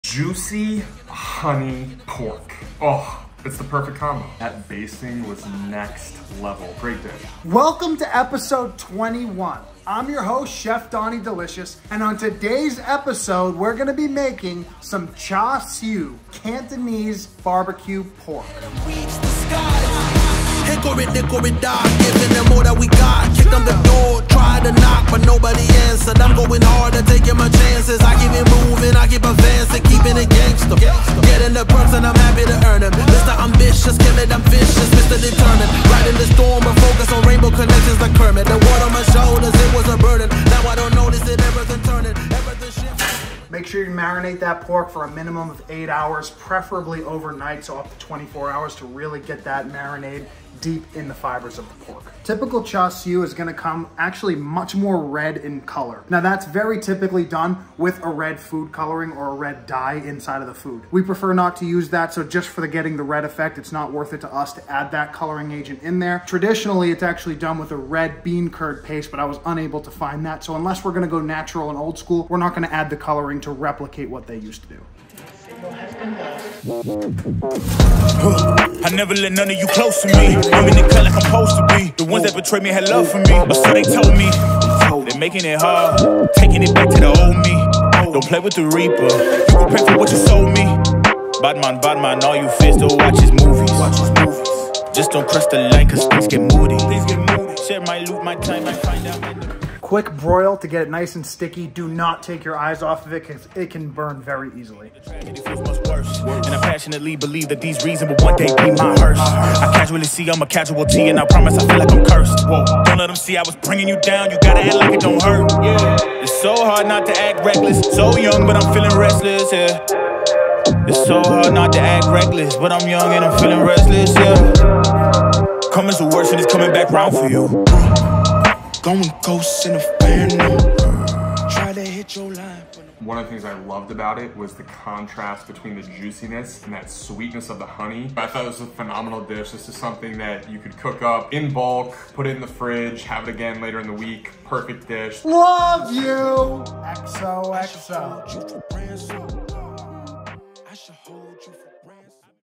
Cantonese BBQ Pork (Char Siu) || Episode 21
Ingredients:

- Pork Butt/Shoulder 3 lbs (Sub: Pork Tenderloin)
- Honey 1/4 Cup & 1 tbsp
- Five Spice Powder 1/2 tsp (AKA Chinese Five Spices Powder)
- White Pepper 1/4 tsp (Sub: Black Pepper, less than half the amount)
- Olive Oil 1/2 tsp
- Shaoxing Rice Wine 1 tbsp (Sub: Sherry Cooking Wine)

Directions:

1. Preheat oven to 475 F
2. Cut pork into long strips about 2.5 inches thick. Do not trim fat
3. Combine honey, salt, five spice powder, white pepper, oil, wine, soy sauce, hoisin sauce, and garlic in a bowl
4. Set some marinade aside (about 2-3 tbsp). Rub pork with the rest of the marinade then cover and refrigerated between 8 & 24 hours.
5. Line sheet pan with foil and wire rack, place pork on rack and about 1.5 cups water in pan below rack.
6. Roast in oven at 475 F for 10 minutes then reduce oven to 375 F but leave the pork in for the next 15 minutes
7. Flip pork upside down then turn it 180 degrees to ensure even cooking and roast for another 15 minutes (add water if gone)
8. Check oven every 10 minutes to ensure its not burning
9. Combine reserved marinade with honey and hot water then reduce to a sticky glaze on stove
10. After 40 minutes of cooking baste both sides of pork then roast 10 more minutes
11. If desired, baste again then broil for final caramelization. Remove pork when it registers at 145 F internally, baste again, and rest for 10 minutes